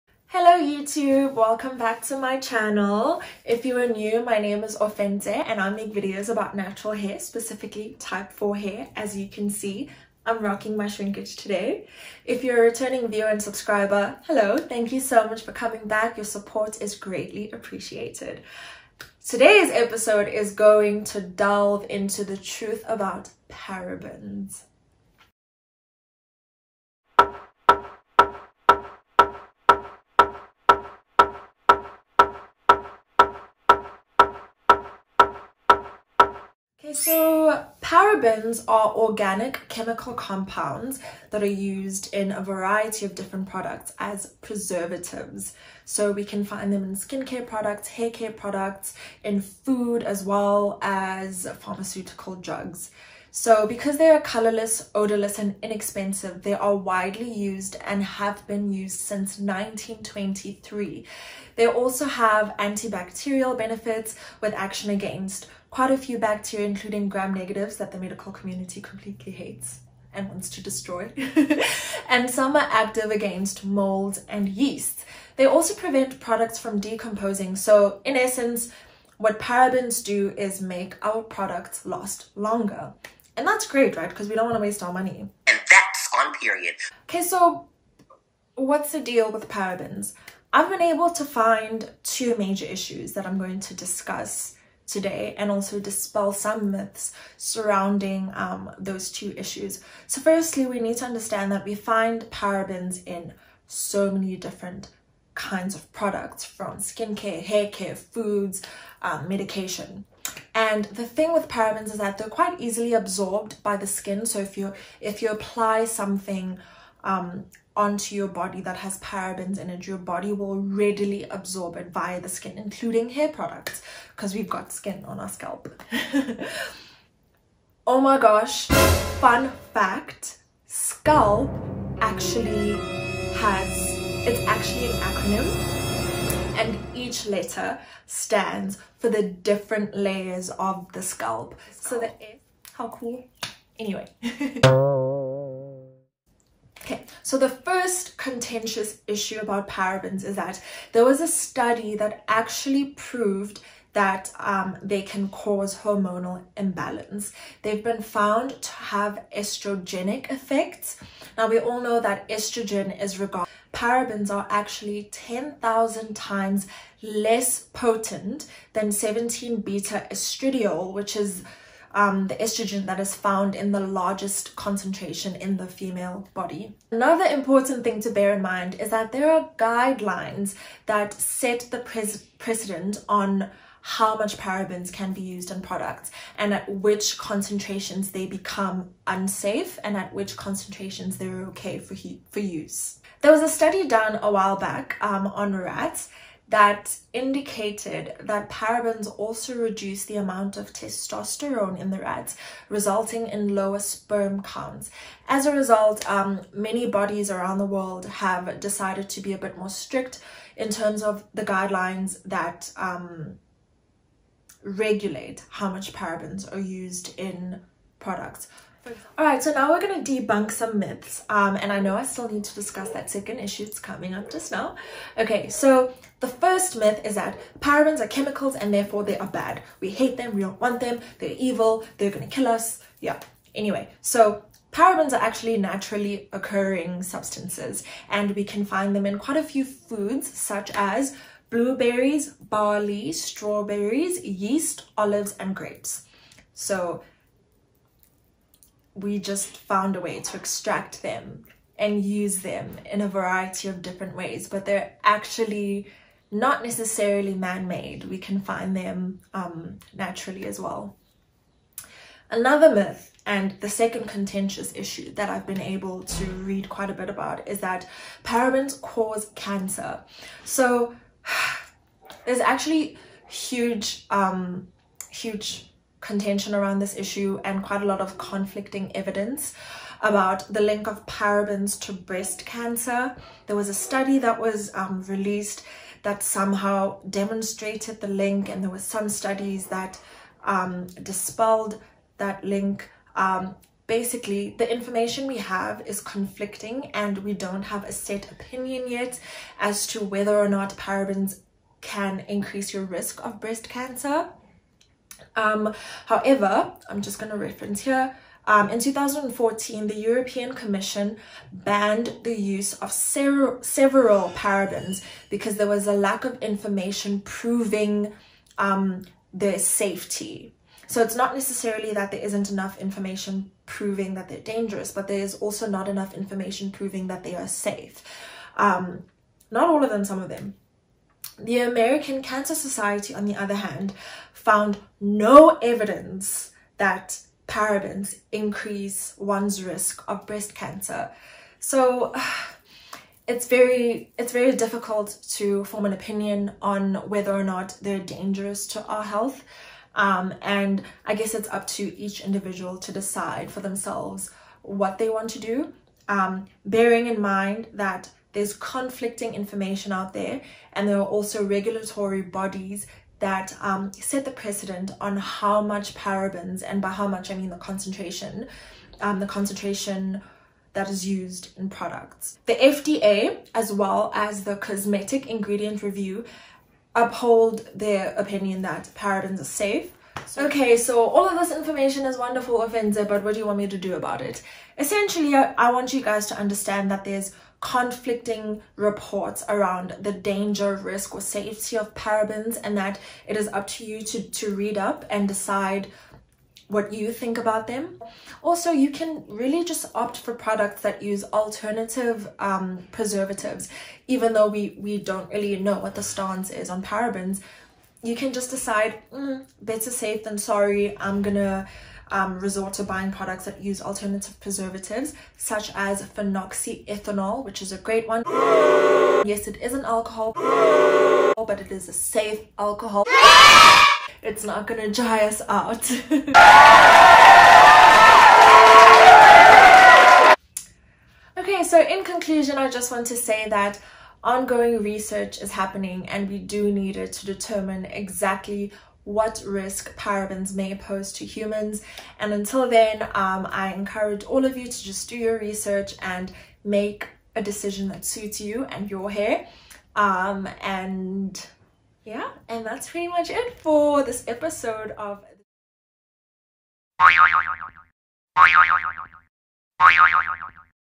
S01E06: The Truth About Parabens || South African Natural Hair YouTuber
Tackling parabens in this extra, Sunday bonus video!